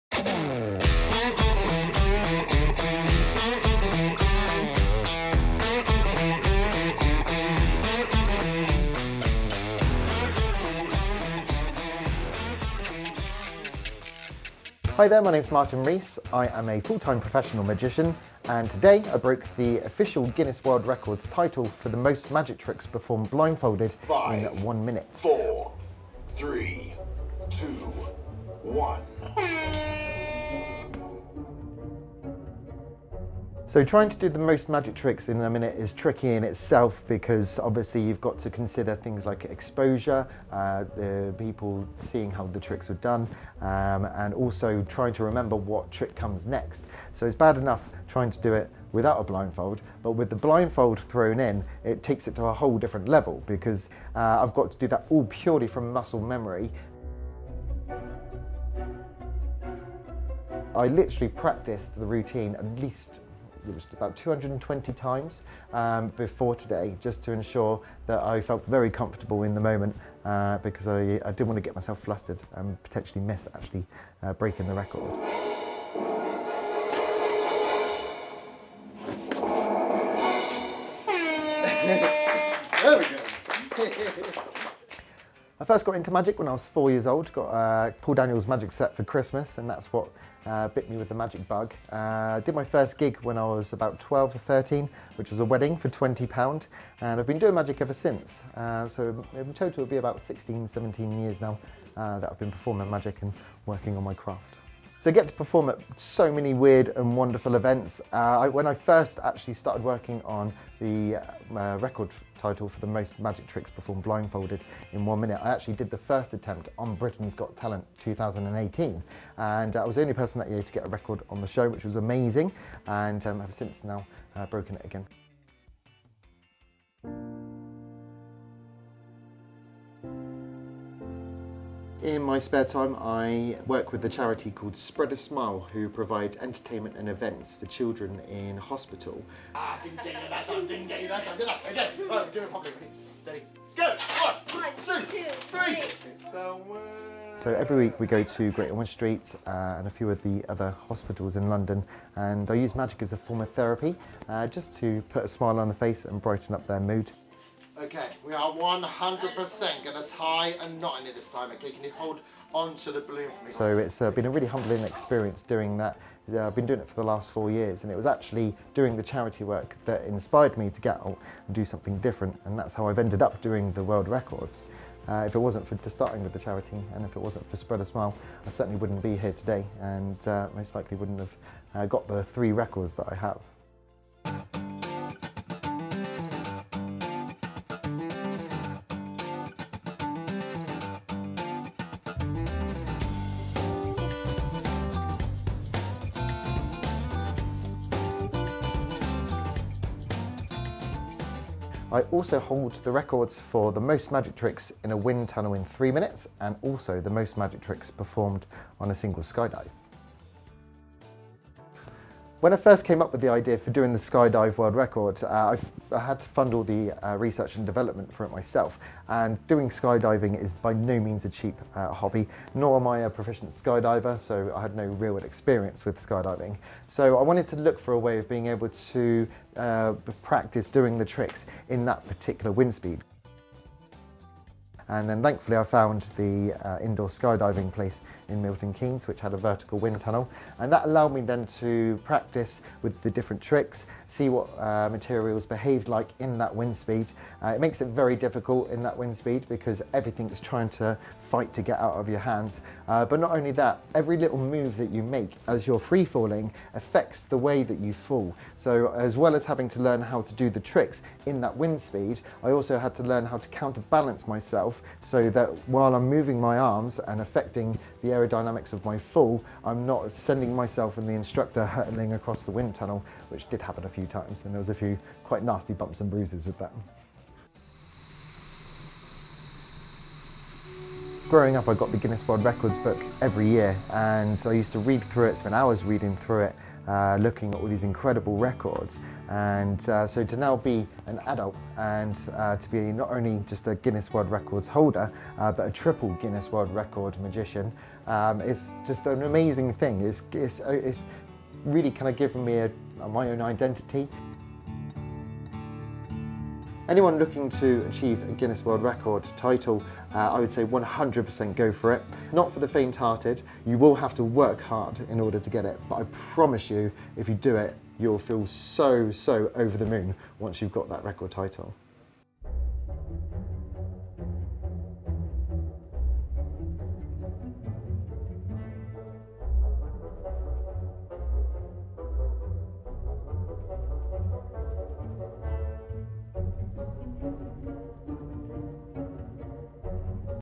Martin Rees: Skydiving Magician! - Guinness World Records
Martin Rees is a magician who loves to take things farther. Already holding the record titles for Most magic tricks in a wind tunnel and Most magic tricks during a skydive, Martin came to GWR HQ to attempt the Most magic tricks in one minute while blindfolded.